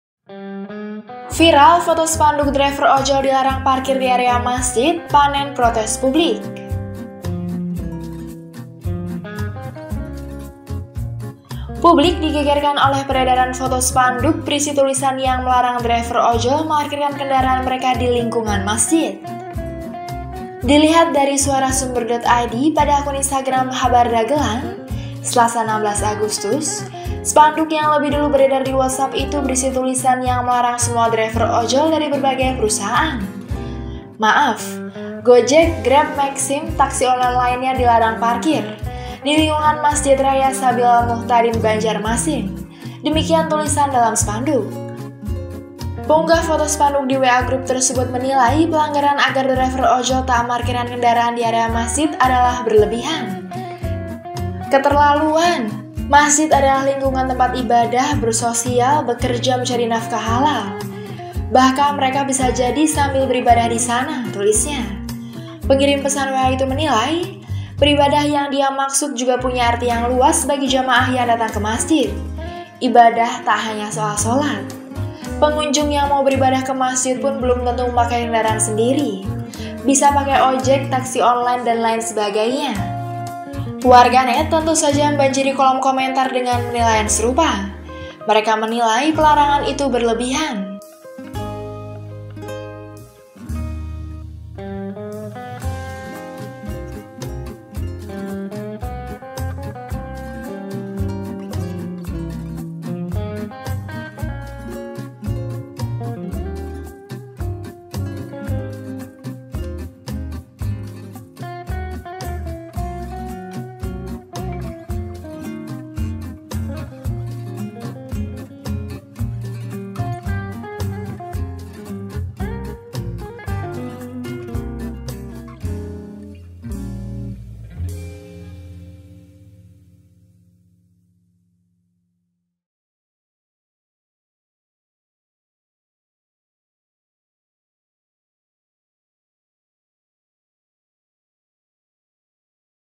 Viral Foto Spanduk Driver Ojol Dilarang Parkir di Area Masjid, Panen Protes Publik
Publik digegerkan oleh peredaran foto spanduk berisi tulisan yang melarang driver ojol atau ojek online memarkirkan kendaraan mereka di lingkungan masjid.

Dilihat SuaraSumbar.id pada akun Instagram @habardagelan, Selasa (16/8/2022), spanduk yang lebih dulu beredar di WhatsApp itu berisi tulisan yang melarang semua driver ojol dari berbagai perusahaan.

"Maaf Gojek-Grab-Maxim-Taksi online lainnya dilarang parkir. Di lingkungan Masjid Raya Sabilal Muhtadin Banjarmasin," demikian tulisan dalam spanduk tersebut.

Penggungah foto spanduk di WA grup tersebut menilai pelarangan agar driver ojol tidak memarkirkan kendaraan di area masjid adalah berlebihan. Selengkapnya dalam video ini.

Pengisi Suara / Video Editor: Anistya Yustika Putri / Bayu Yunianto